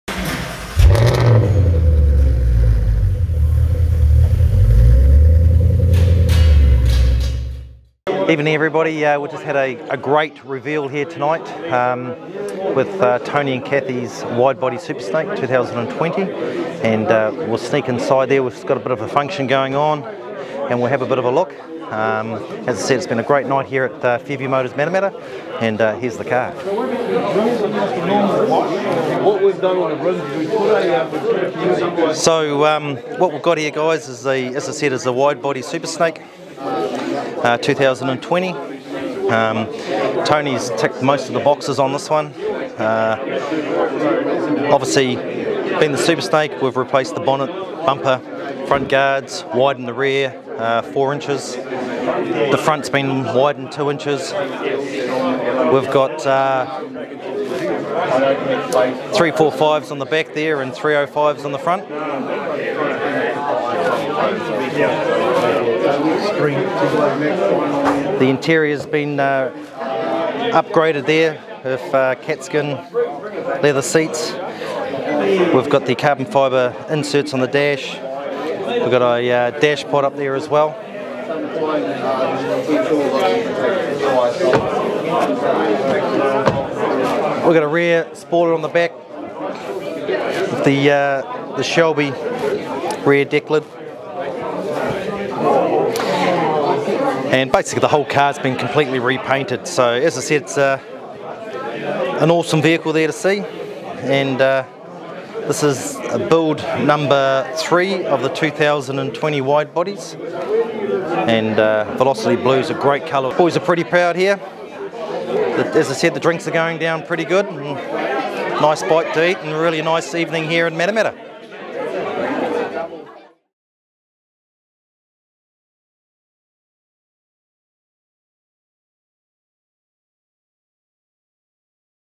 Shelby Wide Body Super Snake Unveiling
On Friday night we had the privilege of unveiling Tony & Kathy's Shelby Wide Body Super Snake at Fairview Motors Matamata. We think its safe to say Tony was very happy and couldn't wipe the smile off his face all night. Congratulations Tony & Kathy and welcome to the Shelby New Zealand family.